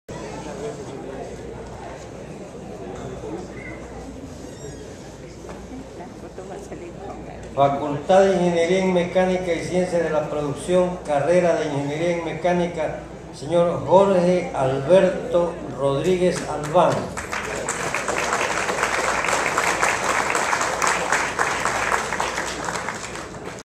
#9 CEREMONIA ESPOL POR SU 51ava era ANIVERSARIO DE CREACIÓN
EL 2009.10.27, EN AULA MAGNA DEL CAMPUS LAS PEÑAS DE LA ESPOL TUVO LUGAR LA CEREMONIA DE PREMIACIÓN Y ENTREGA DE MENCIONES HONORÍFICAS
A PROFESORES, ESTUDIANTES Y TRABAJADORES POLITÉCNICOS CON MOTIVO DEL QUINCUAGÉSIMO PRIMER ANIVERSARIO DE CREACIÓN